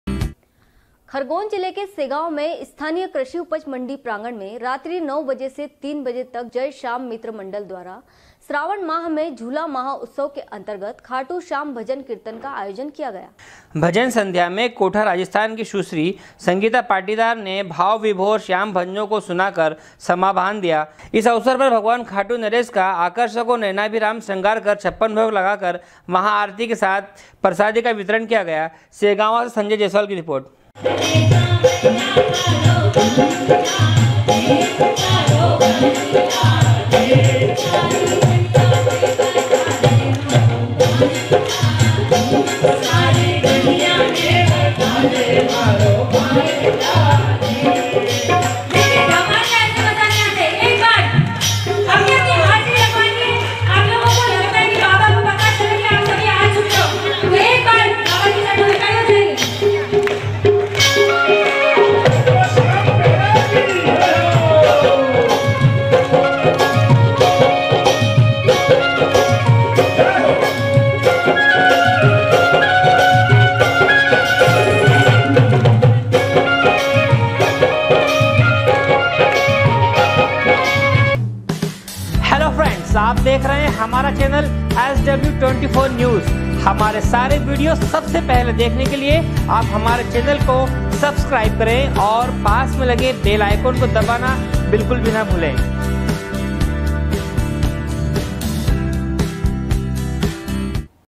sw24news segawa, 29.07.2019
खरगोन जिले के सेगांव में स्थानिय कृषि उपज मंडी प्राँगण मे रात्रि 9 बजे से 3 बजे तक जय श्याम मित्र मंडल द्वारा श्रावण माह मे झुँला महोत्सव के अन्तर्गत खाटु श्याम भजन कीर्तन का आयोजन किया गयाsw24news